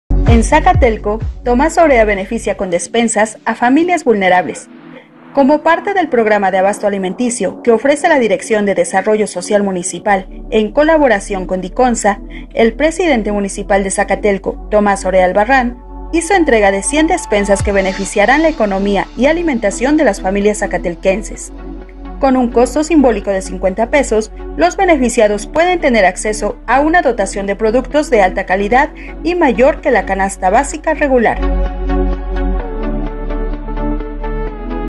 EN ZACATELCO TOMAS OREA BENEFICIA CON DESPENSA
Zacatelco, Tlax. 23 de abril de 2019

EN ZACATELCO TOMAS OREA BENEFICIA CON DESPENSAS A FAMILIAS VULNERABLES

• Como parte del programa de abasto alimenticio, que ofrece la Dirección de Desarrollo Social Municipal, en colaboración con DICONSA

Este, es un apoyo más que la presente administración realiza para acercar los programas a las familias, que gracias a las gestiones que el alcalde municipal ha hecho para mejorar la calidad de vida de los habitantes, se han logrado también entregar otros beneficios como: piso, láminas, techo, calentadores solares y aves de traspatio.

Es importante puntualizar, que a través del convenio con DICONSA, en Zacatelco se mantendrá el precio de las despensas y además aumentarán en un producto, sumando con ello, 13 productos en total, que están elegidos por sus contenidos energéticos para beneficiar la nutrición de la población.

Estos programas, son parte de la oferta de la Dirección de Desarrollo Social, que dirige Emilio de la Peña Aponte, quien por instrucciones del Alcalde Orea Albarrán, ha promovido estos apoyos, con la gente de alta vulnerabilidad que vive en el municipio de Zacatelco.